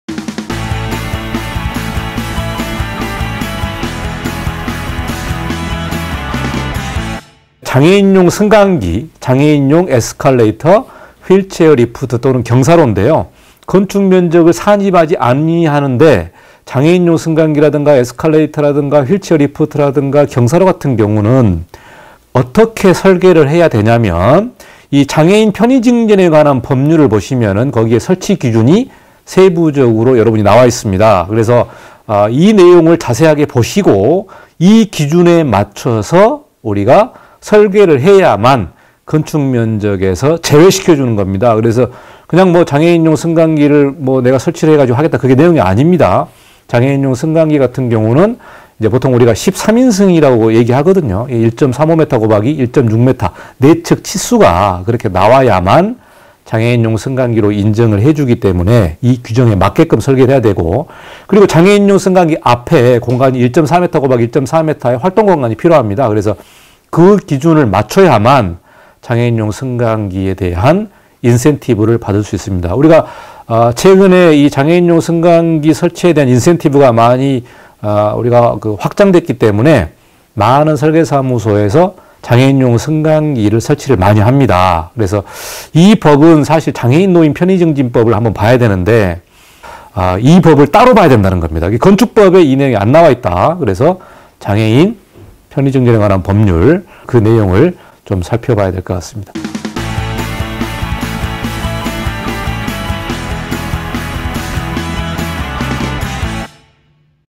장애인 엘리베이터 인센티브 건축사 이관용 건축가 건축설계 (주)오픈스케일건축사사무소
장애인용 엘리베이터 면적 인센티브 받으려면?
- 장애인용 엘리베이터 설계기준
- 인센티브 적용여부
건축사 이관용 건축가 건축설계
(주)오픈스케일건축사사무소 대표